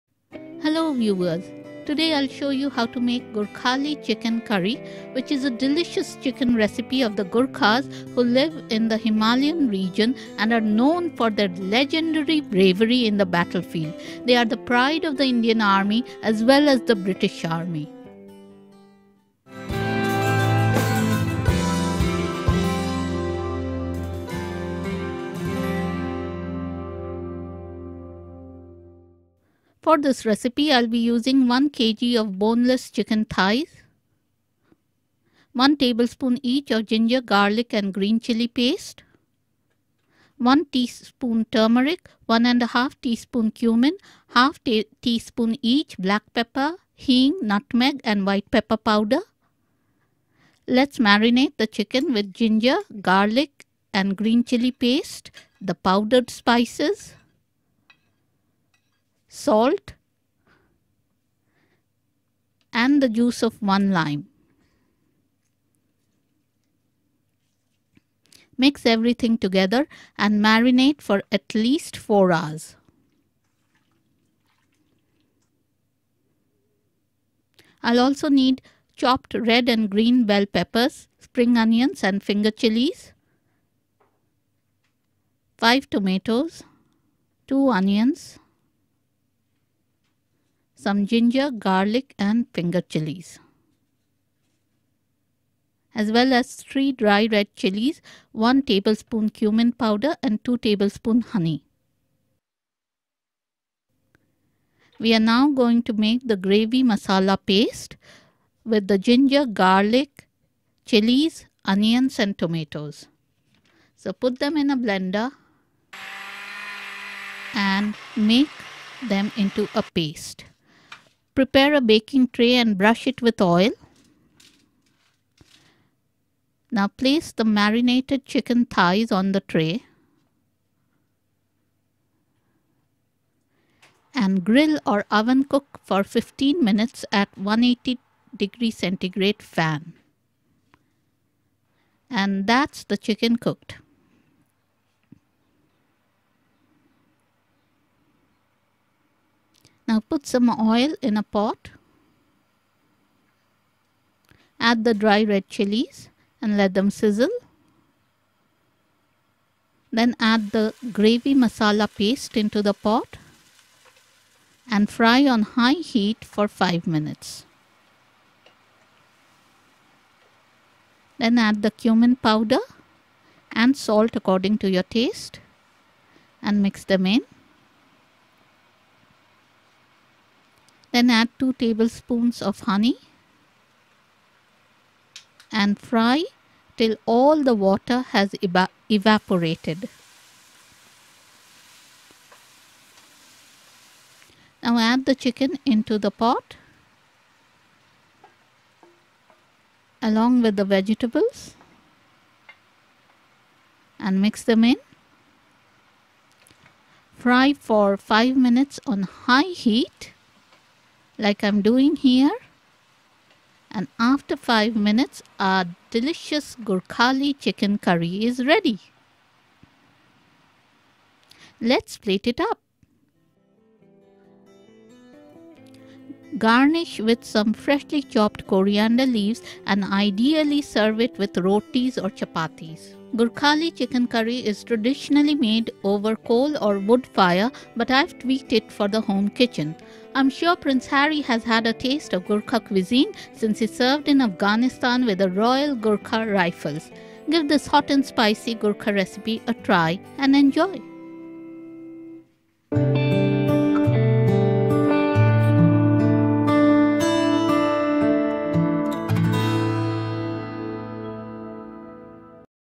GURKHALI CHICKEN CURRY II A spicy and flavorful chicken curry from the Himalayas
Gurkhali Chicken Curry is a chicken recipe of the legendary Gurkha community from the Himalayan region of Nepal and Eastern India. The Gurkha community is known for their legendary bravery on the battlefield and are the pride of the Indian as well as the British Army. Traditionally this recipe is made over a coal or wood fire, but I have tweaked it for the home kitchen. Give this delicious Gurkha Chicken recipe a try!

Composer: Sound Idea
Label: Sound Idea